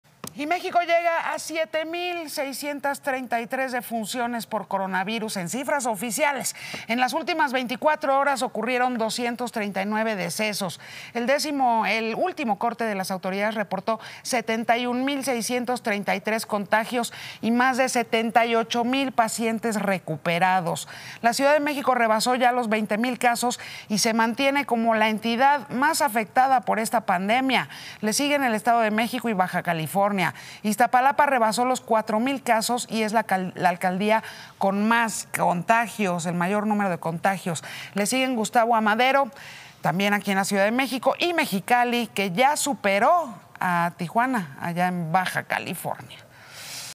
Covid-19 en México: defunciones, casos positivos y recuperados | Noticias con Yuriria Sierra
Suman en México 7 mil 633 fallecimientos por coronavirus y se reportan 71 mil 105 casos positivos, la CDMX sigue siendo la entidad más afectada.